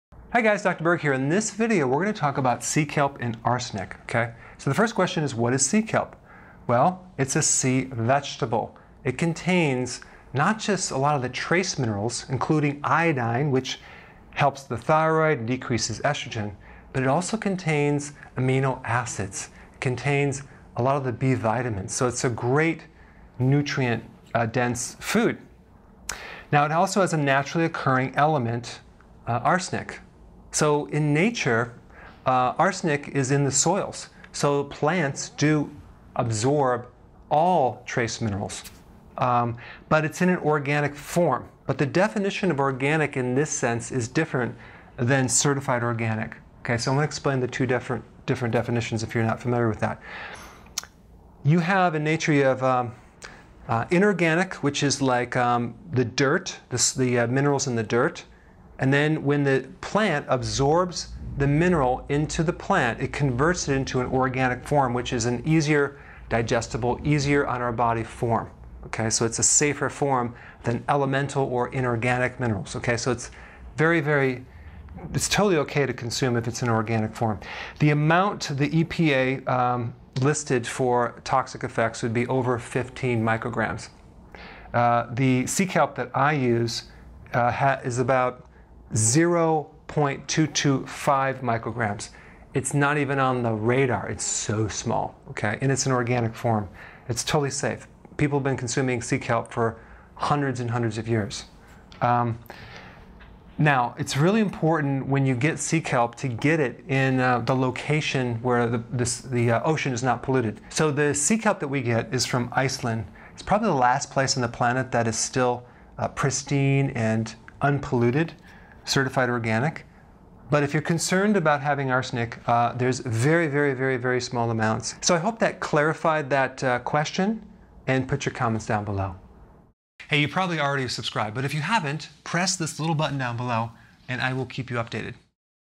Is Arsenic Dangerous in Sea Kelp? – Dr.Berg
Dr. Berg answers the question "Is Arsenic dangerous in sea kelp?" Sea kelp contains an excellent source of iodine which is good for the thyroid and decreases estrogen and vitamin B. It also has a naturally occurring element organic form of arsenic. When it is an organic mineral, it is easier for your body to digest it rather than inorganic.

It is considered toxic to the body when it is over 15mg per EPA, but the one we use is  0.225mg. The Sea kelp we get is from Iceland and is certified organic.

Watch the video to learn the different forms you can find arsenic in, why sea kelp has it in the first place and why it is safe for you in eating sea kelp. Understand the exact amounts that are safe and what you need to be aware of.